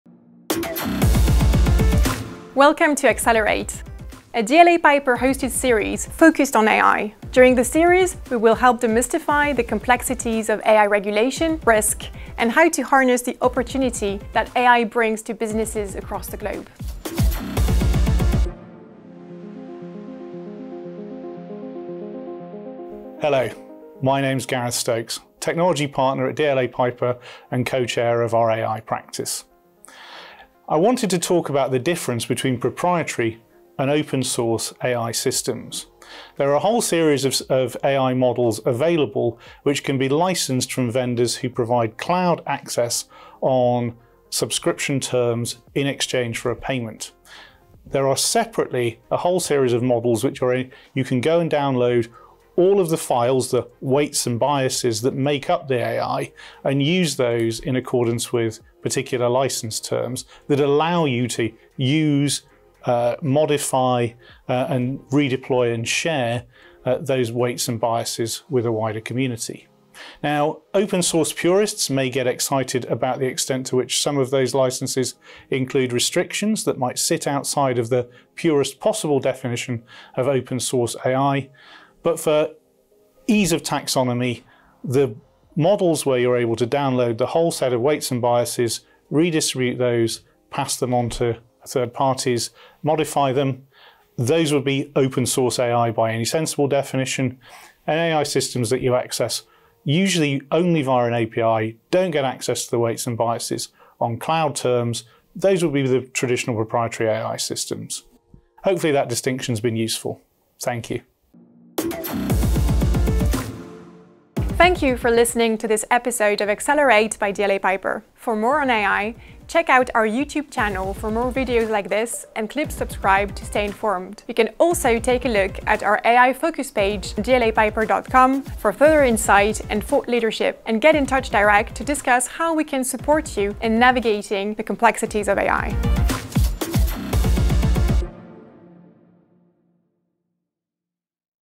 Accelerate: 06 - What is the difference between proprietary and open source AI systems?
What should you consider when choosing an AI model? Gareth Stokes, Global Co-Chair of our AI group discusses the differences between proprietary and open source AI systems in our latest Accelerate episode.